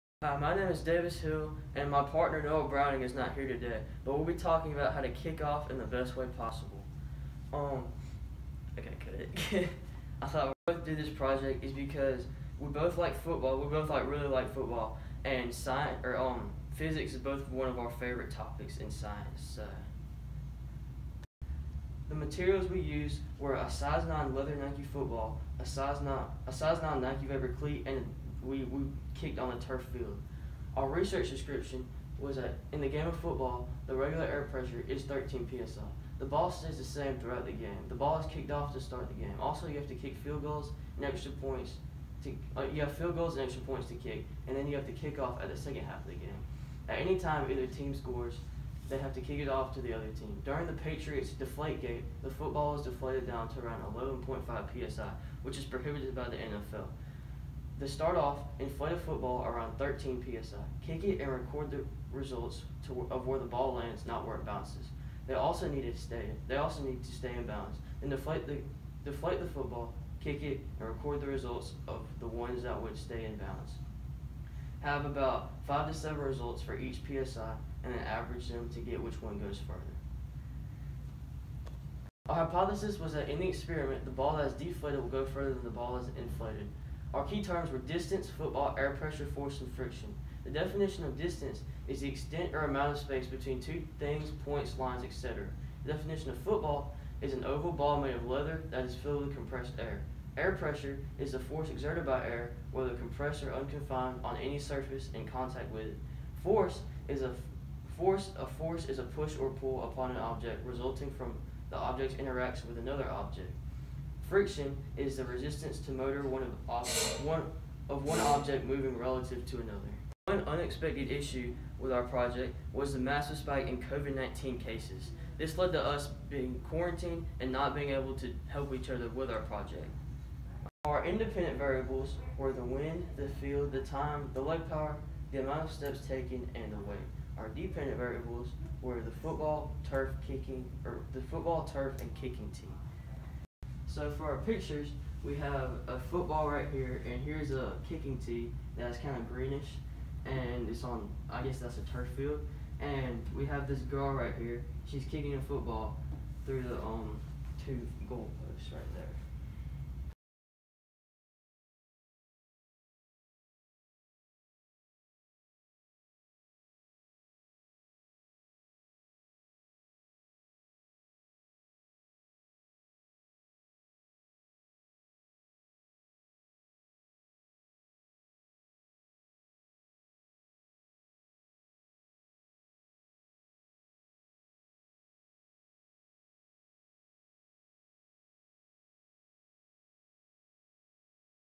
Kicking Off Best Possible: Does air pressure in footballs affect the distance of a kicked football?
Davis Hill and Noah Browing from William James MS. Regional SEF